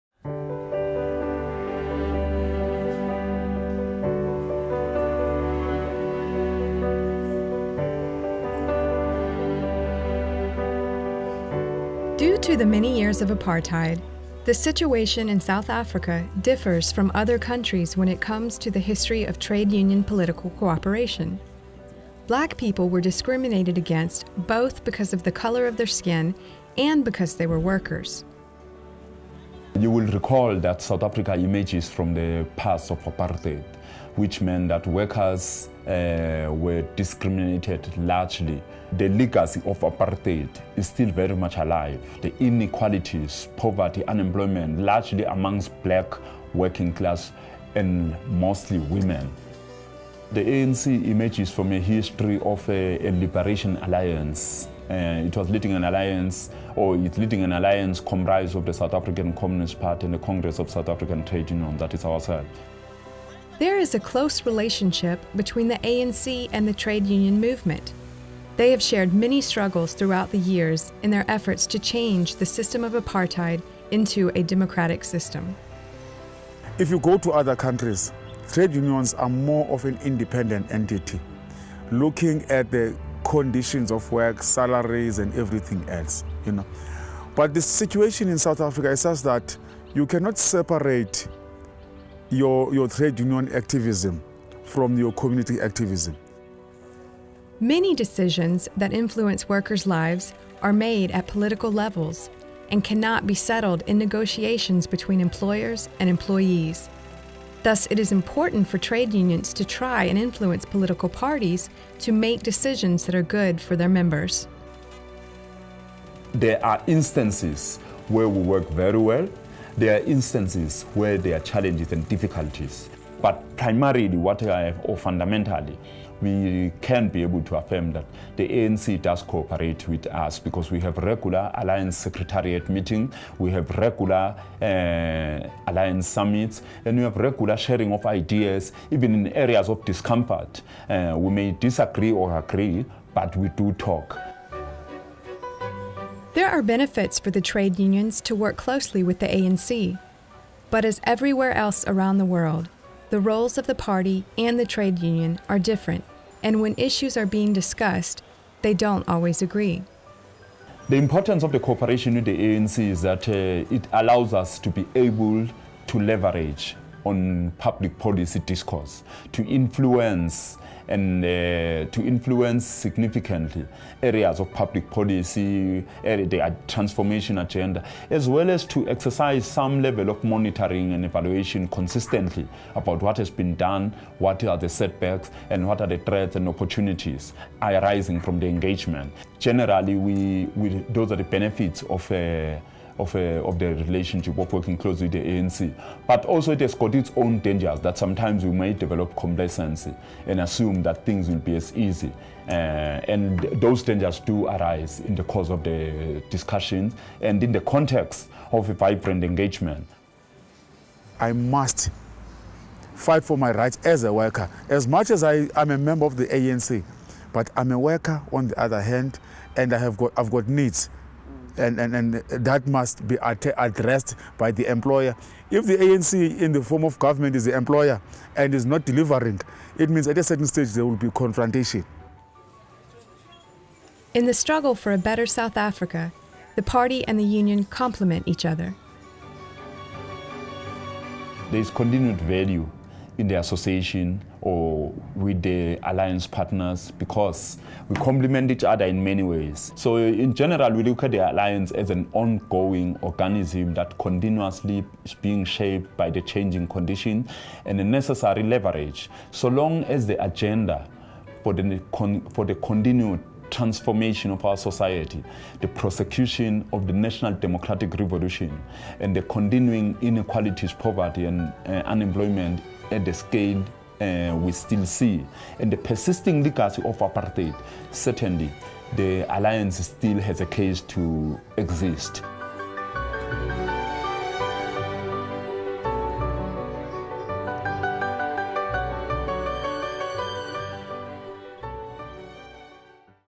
SYDAFRIKA: Facklig-politiskt samarbete
Innan apartheidsystemet föll i Sydafrika var den fackliga centraloragnisationen COSATU en del av befrielserörelsen ANC. I dag - när ANC är ett regeringsparti - är rollerna annorlunda.

- Det finns tillfällen då vi arbetar mycket bra ihop, men det finns också fall som innebär utmaningar och svårigeheter, säger Bongani Masuku, internationell sekreterare i COSATU.

Filmen (4:54) är producerad av Palmecentret 2012. Medlemsorganisationer och andra intresserade är välkomna att sprida denna vidare och att använda den i sitt informationsarbete.